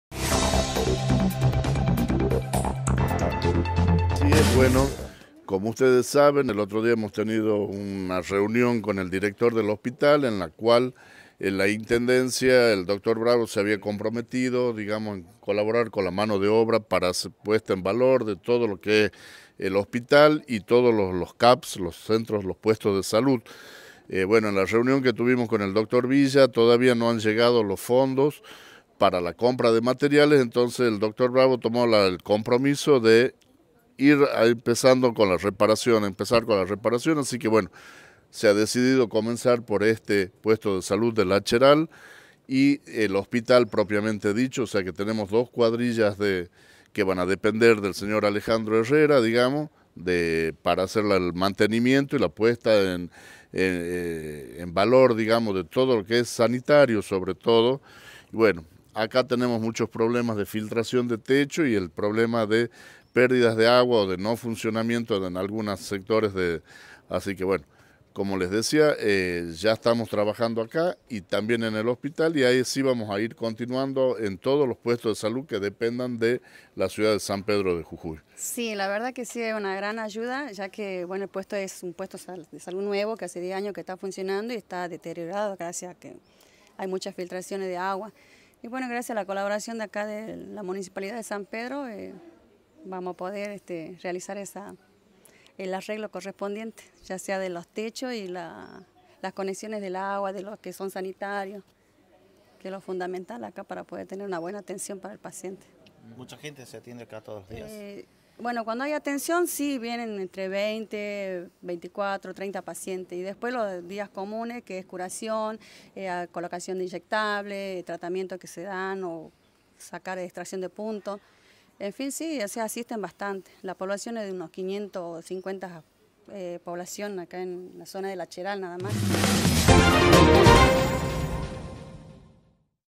Se realizarán mejoras edilicias en el puesto de salud de la localidad de El Acheral
Hugo Samman - Srio. O.S.P. y Medio Ambiente
Mabel Lucena - Enfermera